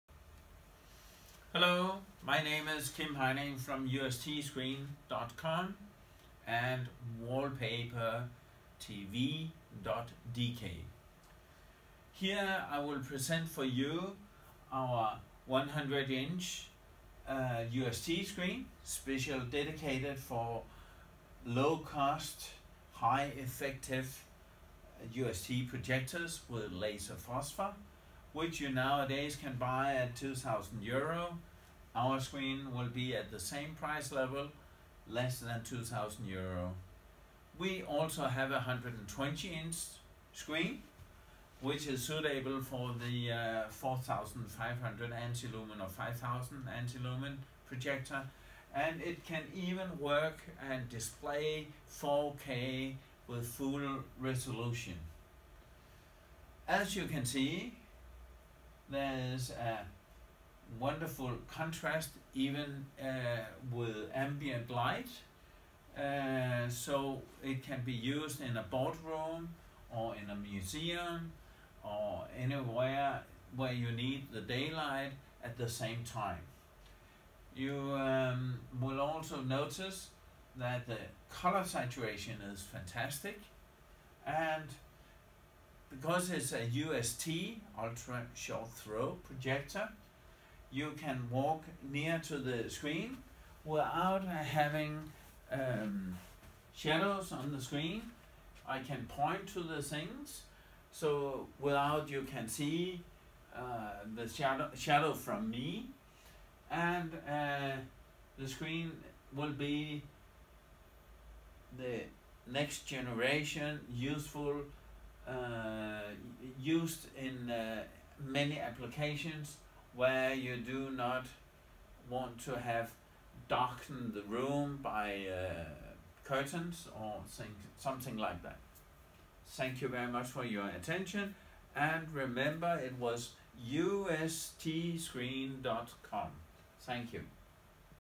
UST screen with Lenticular Surface 100" and 120"
UST-screen.com is videoprojection screens dedicated for UST-projectors with lenticular lenses (like fresnel lenses) for use in ambient light. The USTscreen deliver wonderfull images even in 4K  with fine color saturation and fantastic black-level - even in daylight. Suitable for MUSEUMS, BOARDROOMS, SPORTBARS.
The screen is VERY COSTEFFECTIVE .